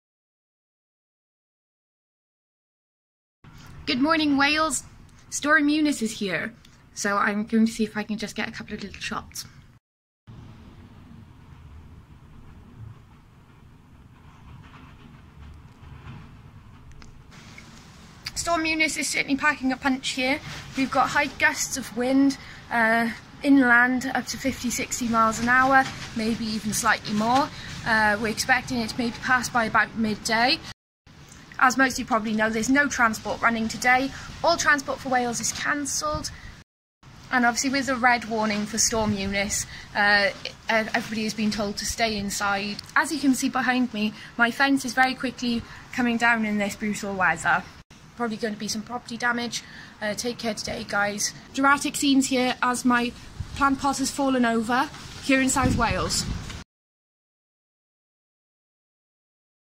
Storm Eunice South Wales Morning Outlook plus footage
This morning is certainly shaping up to be a stormy one for sure.